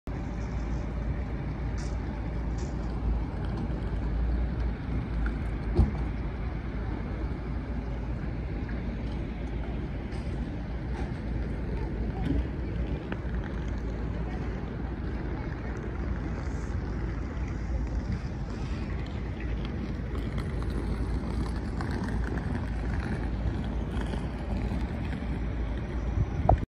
Остановка автобуса Аэроэкспресс в Аэропорту Домодедово (до метро Домодедовская)
Остановка автобуса Аэроэкспресс в Аэропорту Домодедово (до метро Домодедовская), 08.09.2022. Температура воздуха +8.